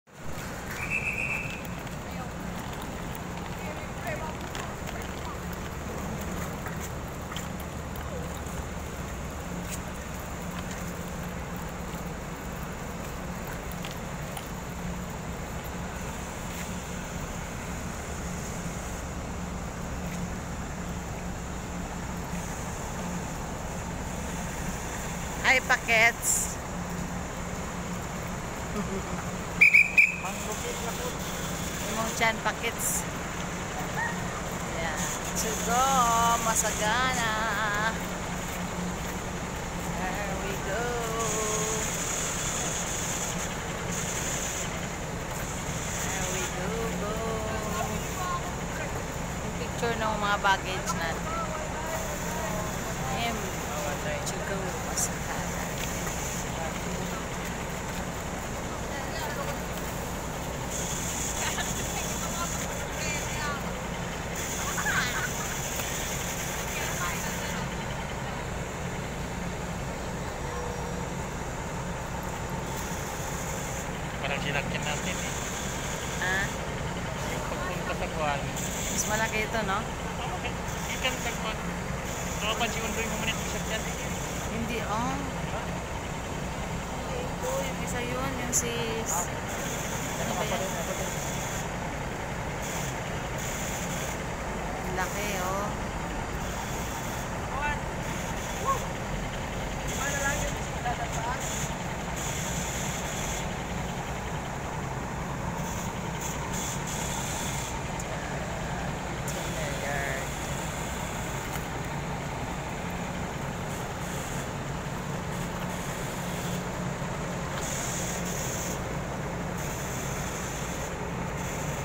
Pang international ang laki ng barko M.V. 2GO Masagana watch and subscribed
M.V. 2GO Masagana biyaheng Manila Cebu CDO and Vice Versa very affordable ang fare and very convenient mga facilities accomodations maganda at malinis may entertainment pa. Sakay Na!!!